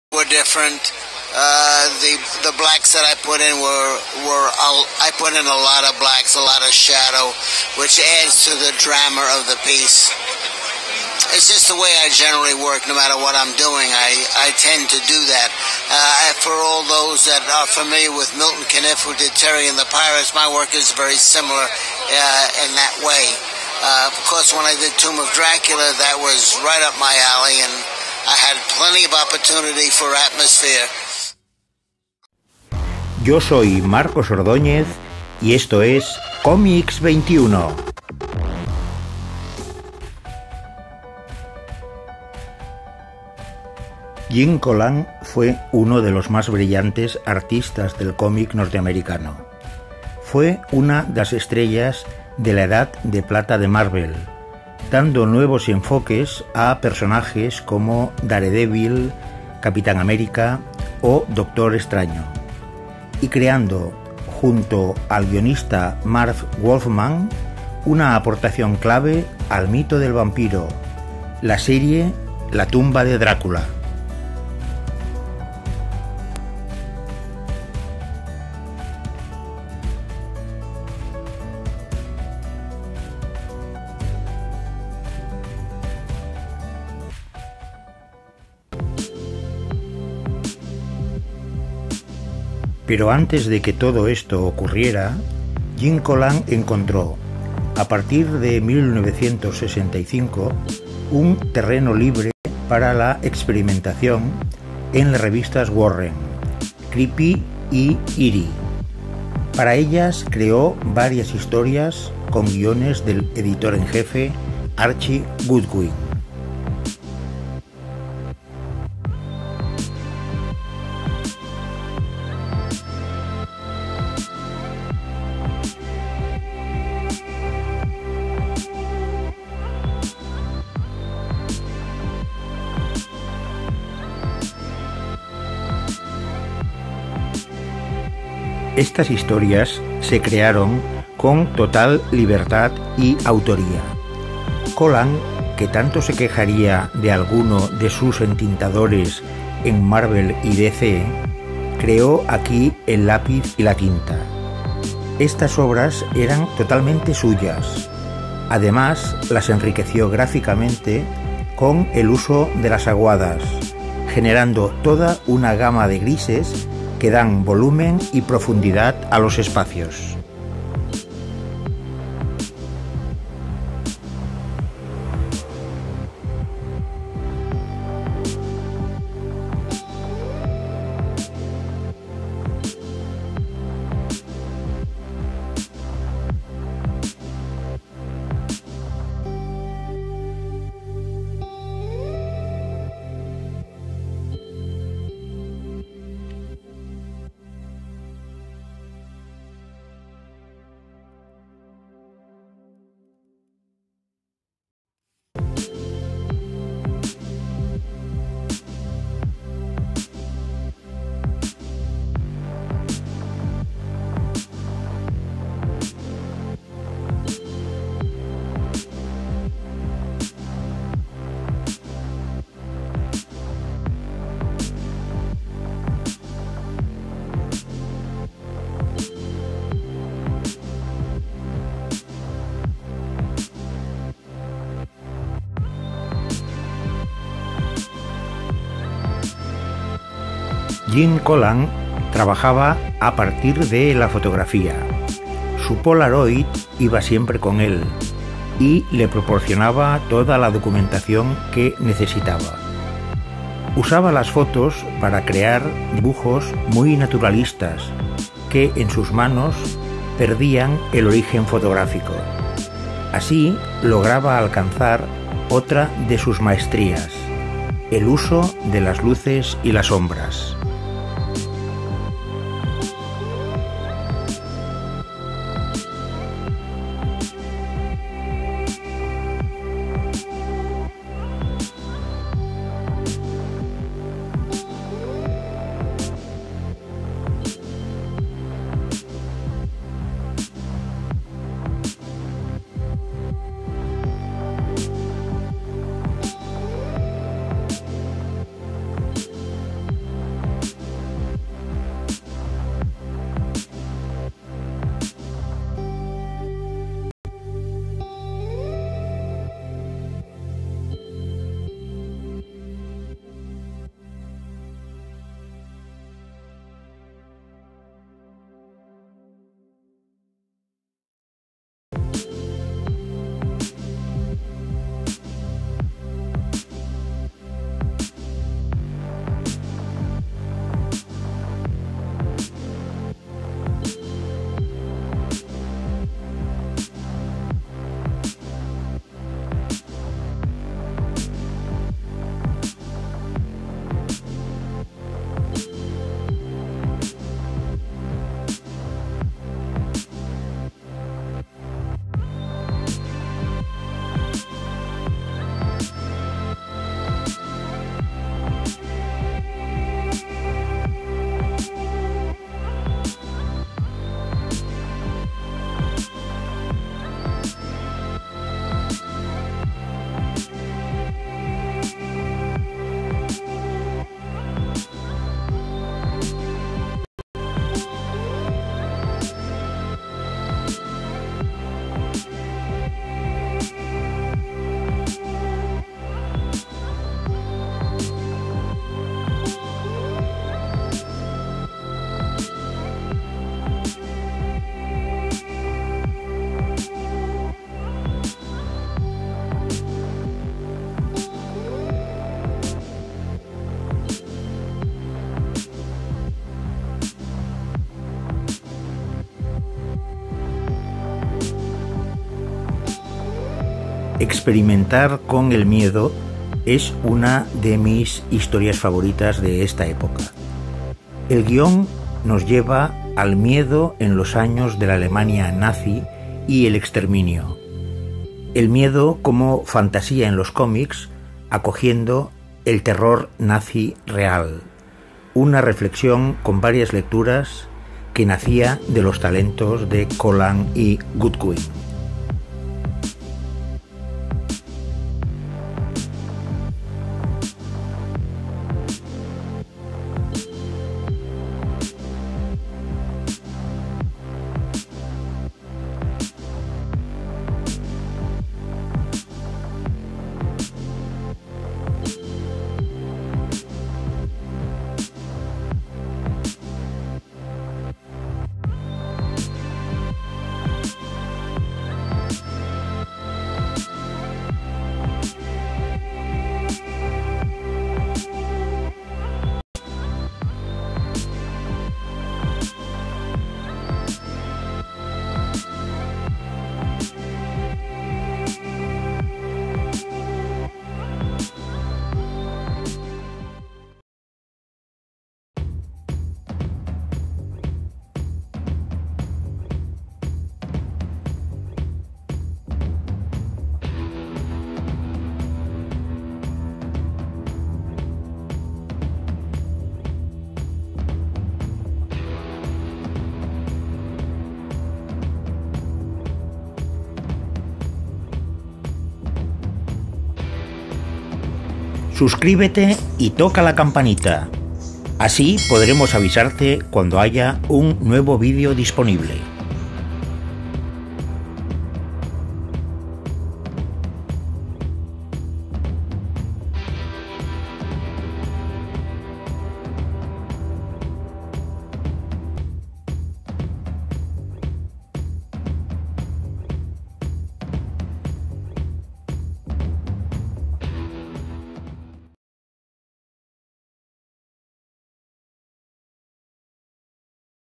Gene Colan Cómics XXI (62)
Gene Colan, fue un magnífico dibujante que desarrollo su trabajo sobre todo en Marvel, en series como Daredevil. Durante unos años colaboró con las revistas Warren, Creepy y Eerie, con historias que creo en absoluta libertad, tanto ideológica como de estilo. Fue él mismo porque todos los trabajos eran completamente suyo, sin entintadores.